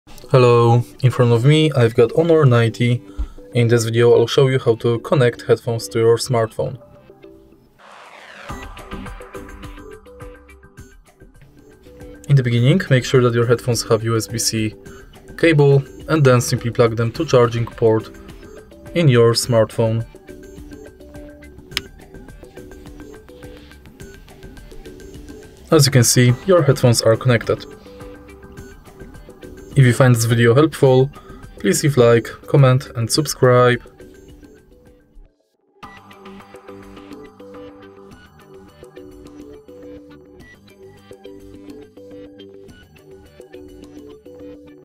How to Connect Headphones in HONOR 90 – Add Headphones
If you'd like to learn how to connect the headphones to your HONOR 90, then here we are coming to show you! In this tutorial, we would like to guide you to a proper section, thanks to which you'll be able to use the connection and pair both devices. So let's follow all shown steps and successfully connect headphoneswith HONOR 90!

How to connect earphones to HONOR 90? How to connect headphones to HONOR 90? How to add headphones in HONOR 90?